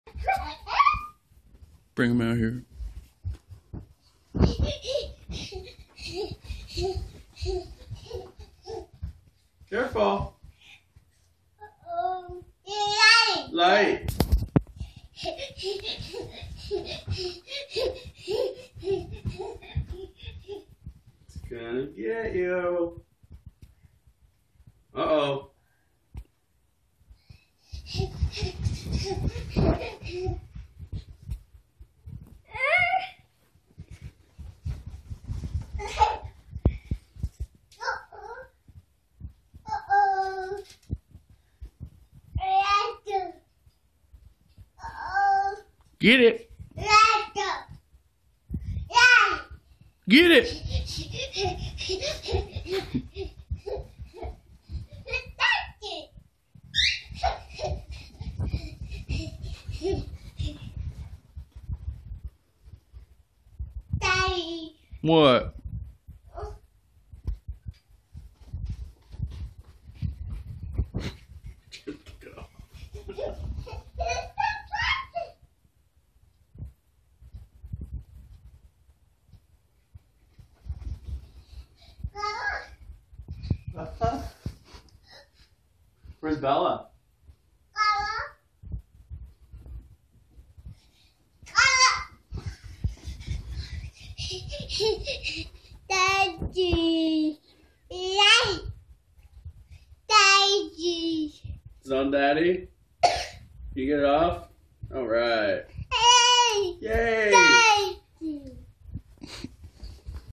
Laser pointer chase lol
Bray trying to find the light